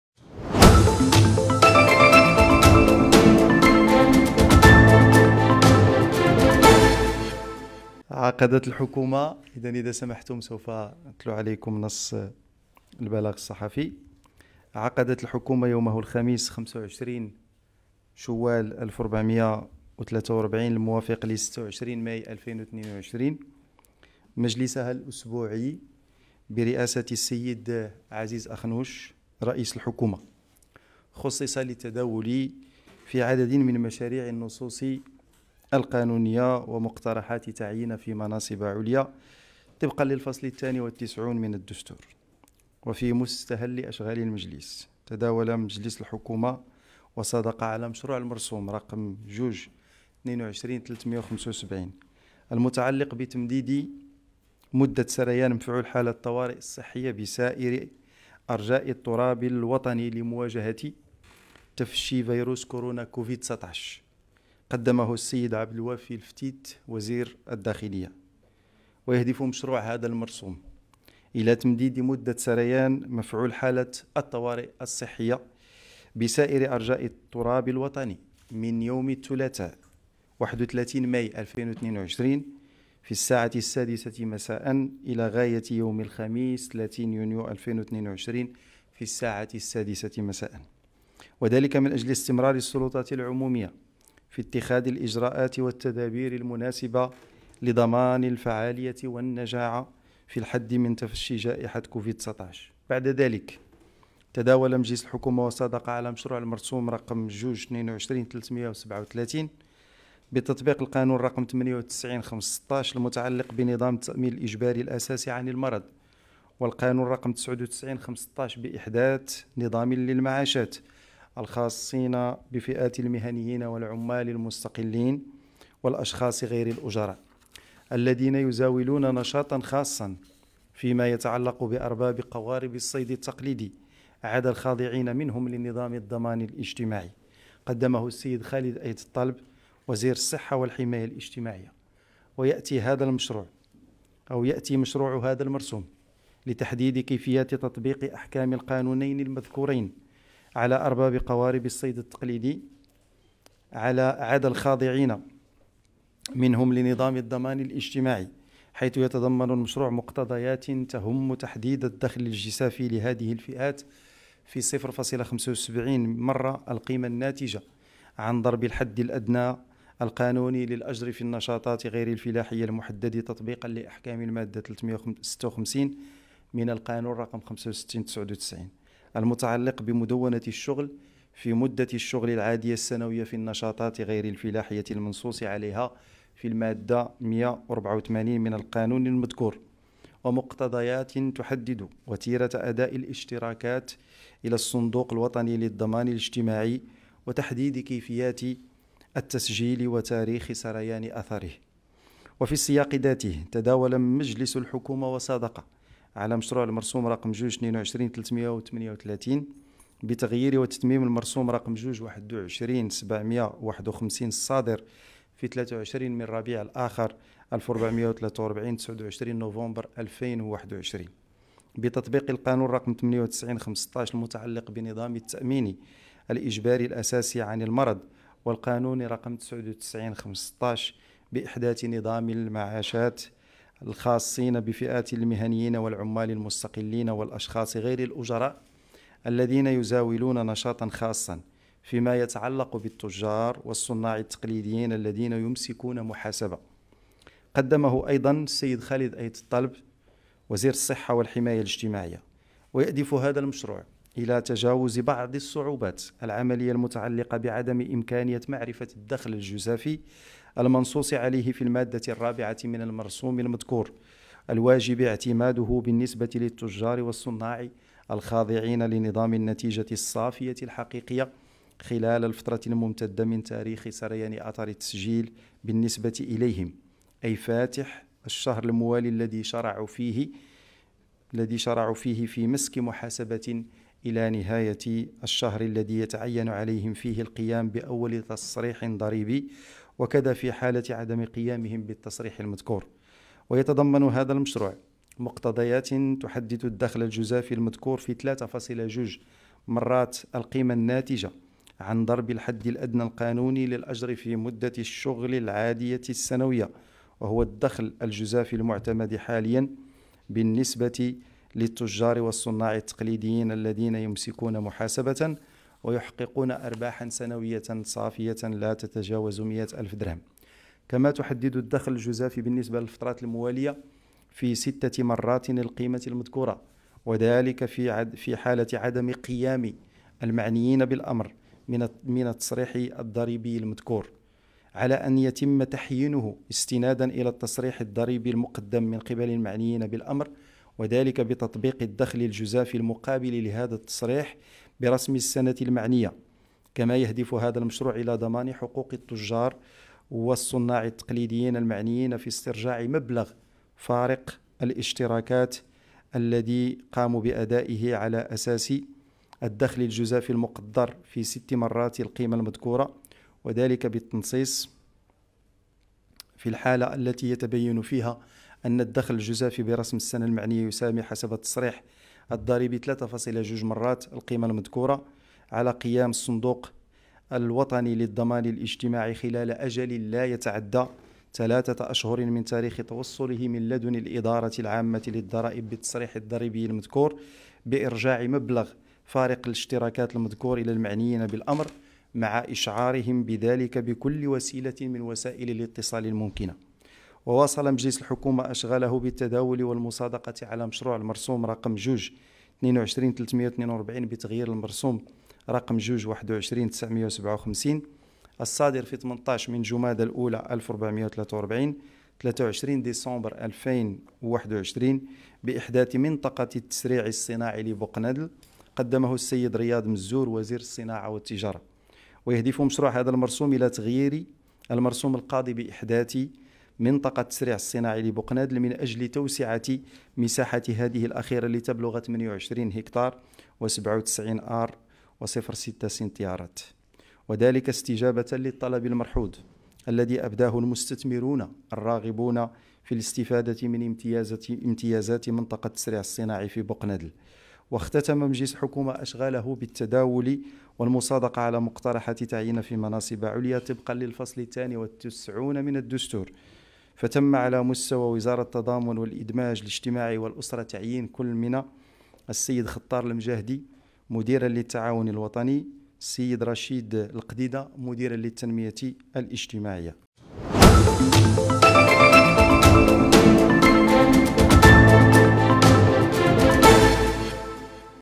هذا ما تداوله مجلس حكومة يوم 26 ماي 2022
في هذا الفيديو ستتعرف على ما تداوله مجلس الحكومة يوم 26 ماي 2022

# أخبار_عاجلة